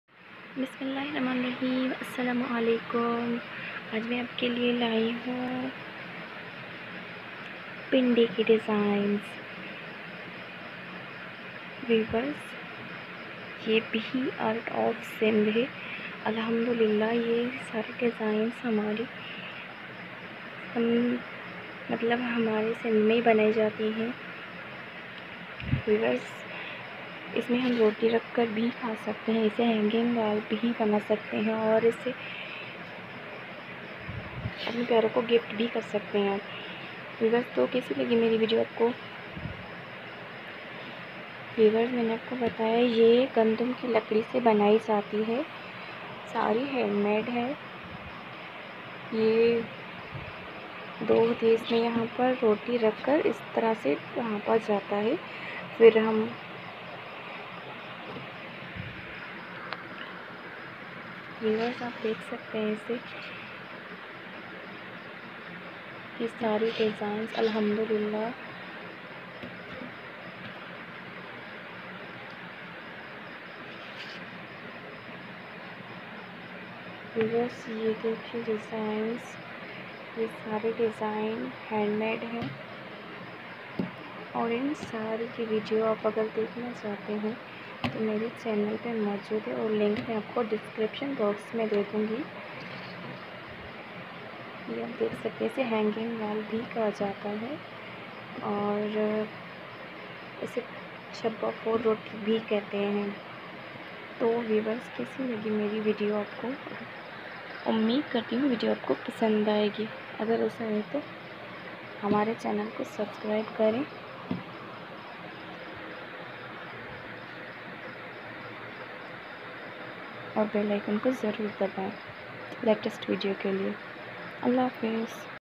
Art of Sindh handmade Wall Decoration Designs for idea By Remedies with M Mahdi
Sallam frienD's

Welcome to Aur Channel Remedies with M Mahdi

Views aj Mai apky liye Lai hun hand made hanging wall Designs
Ye Gandum ke lakre sy bnai gai hai or is py goty Ka use Kia Gaya hai
Ummid karte Hain video aapko pasand aaegi